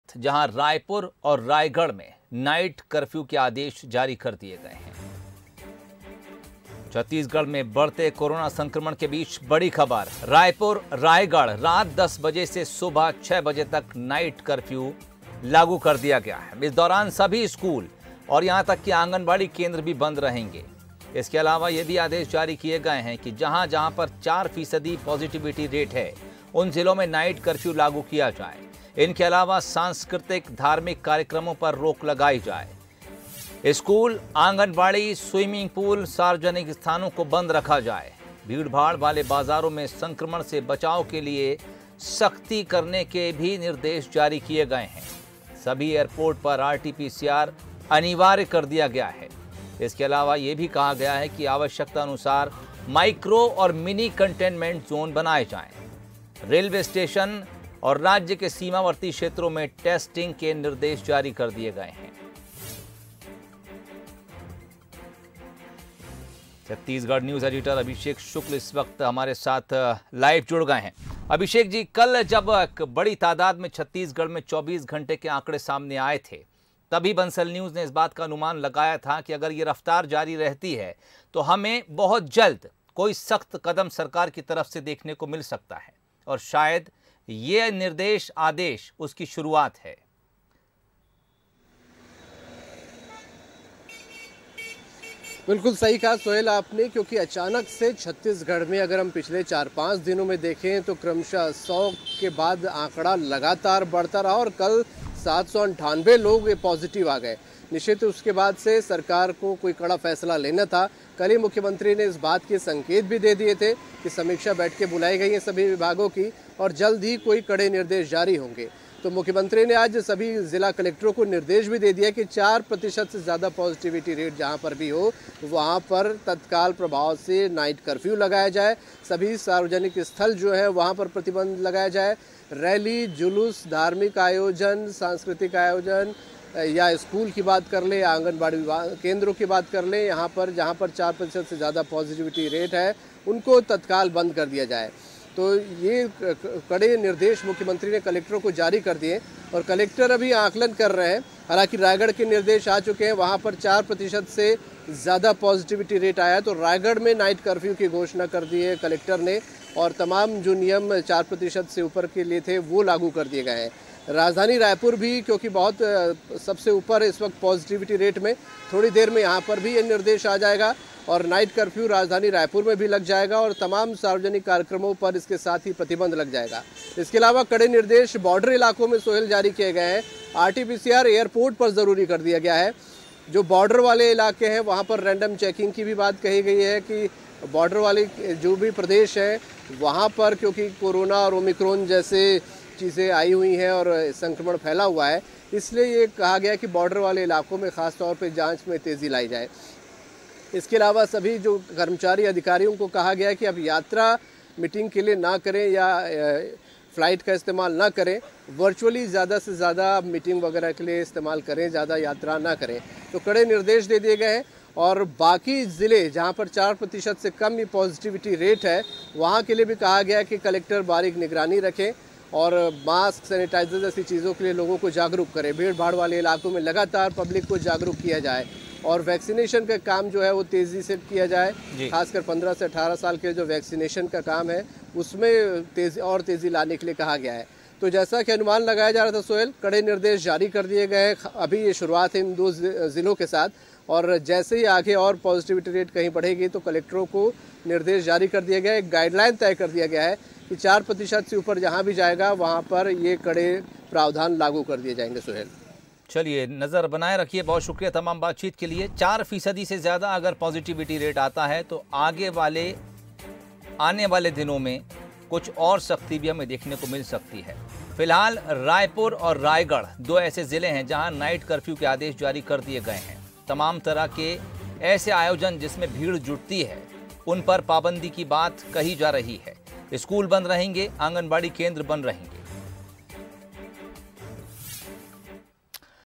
रायपुर, रायगढ़ में नाइट कर्फ्यू,जुलूस,रैलियों के आयोजनों पर रोक, स्कूल और आंगनबाड़ी केंद्र बंद
छत्तीसगढ़ की राजधानी रायपुर और रायगढ़ में कोरोना के बढ़ते संक्रमण को देखते हुए... जिला प्रशासन ने रात 10 से सुबह 6 बजे तक नाइट कर्फ्यू की घोषणा की है.. इसको देखते हुए सभी स्कूल और आंगनबाड़ी केन्द्र बंद रहेंगे..